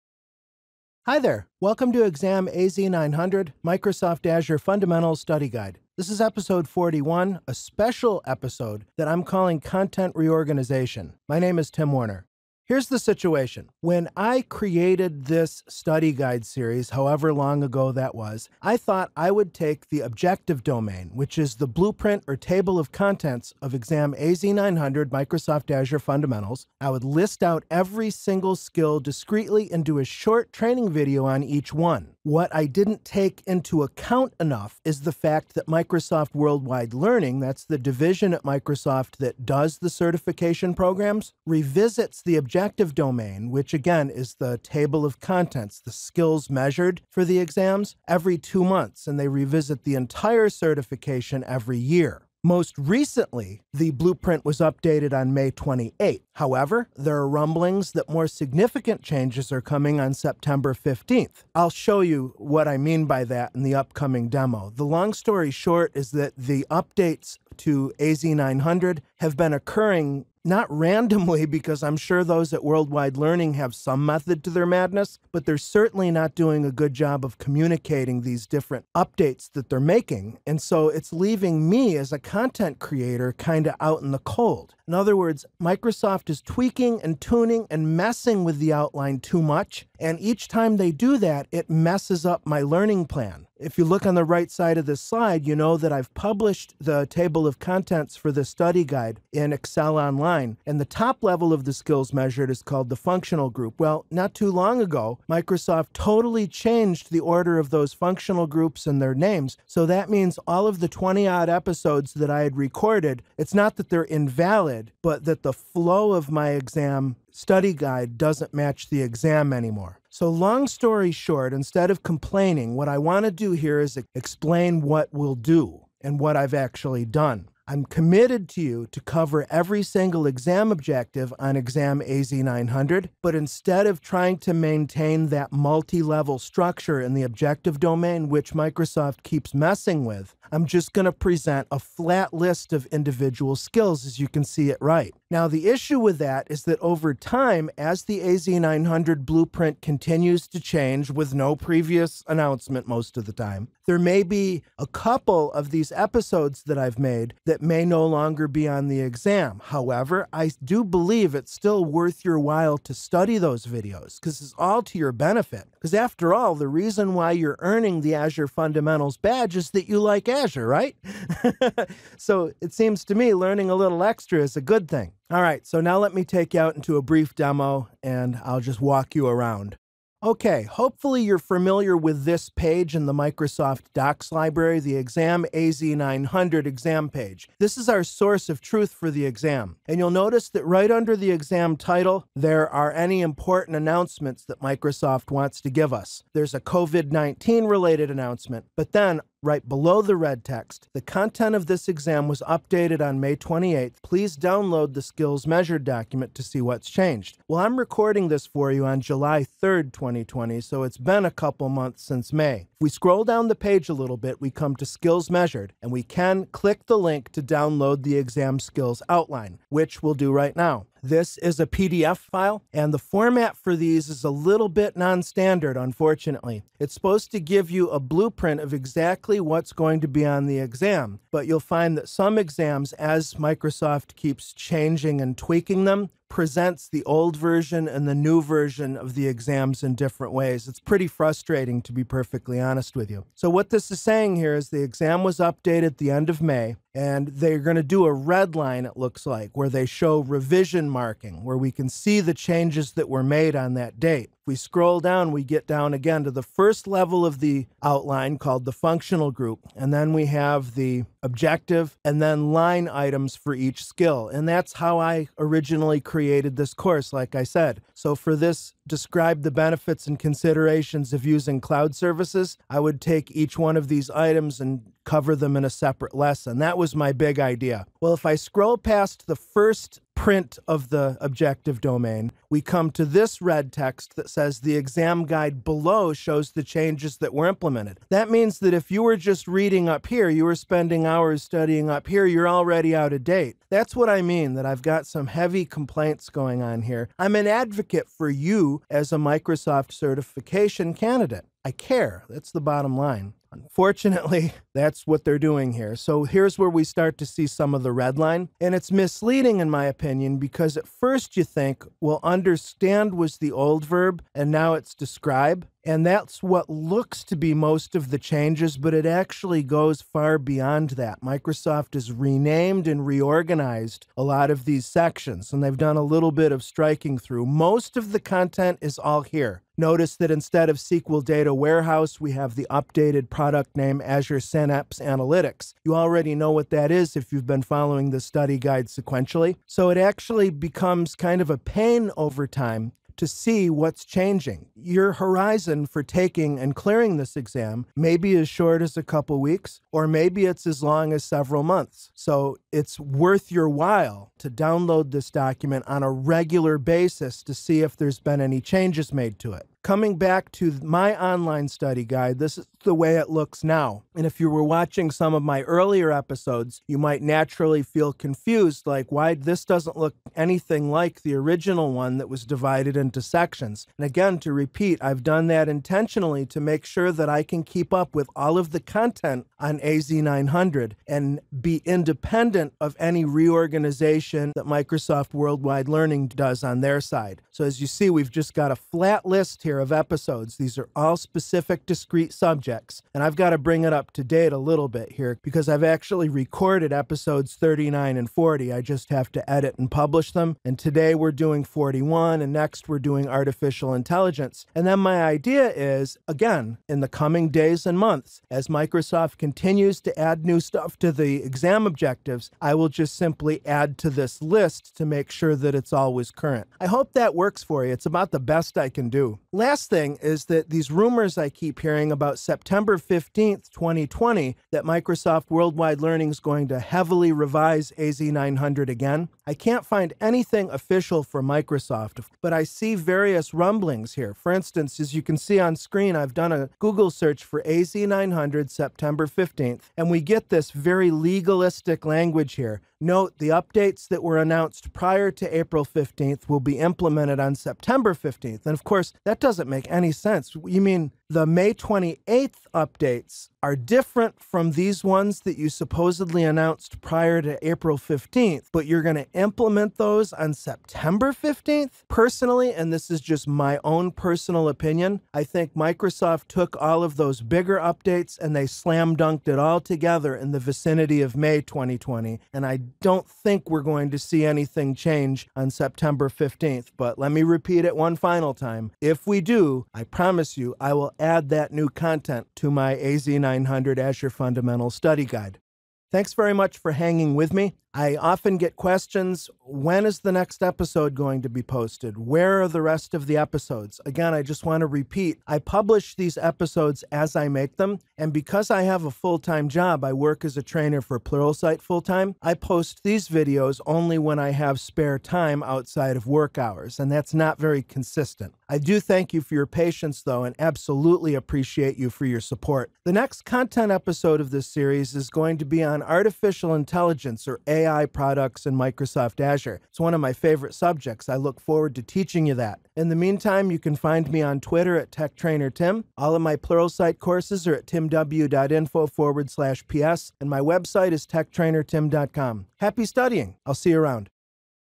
Exam AZ-900 MS Azure Fundamentals Study Guide Episode 41: Content Reorganization
This episode is less of a lesson and more of a rant about MS Worldwide Learning's frequent and mostly unannounced Azure exam modifications.